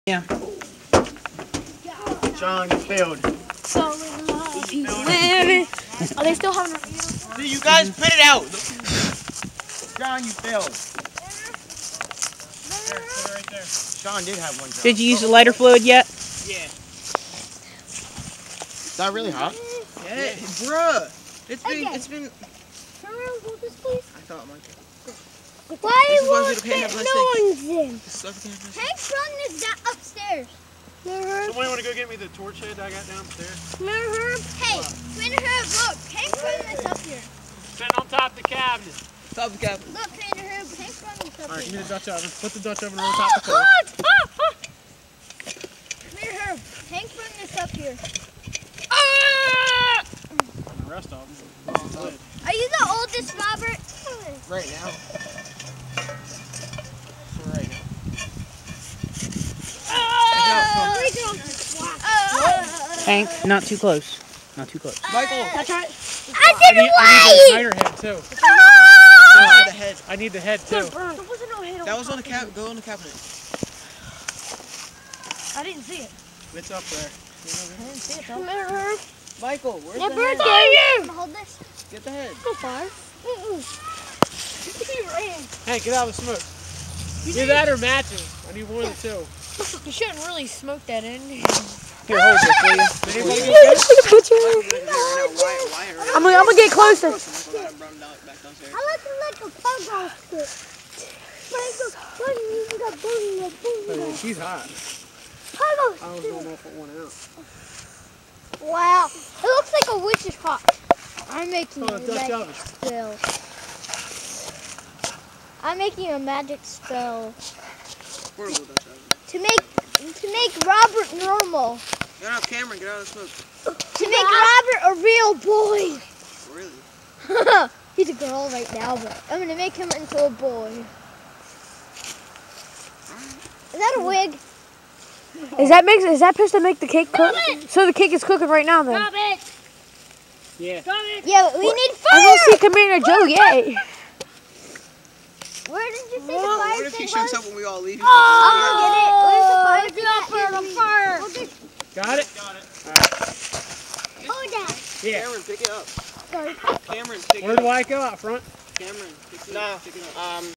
Royal Rangers Campfire (Dutch oven)
We're baking a cake in a Dutch oven, teaching our boys the dutch oven cooking merit.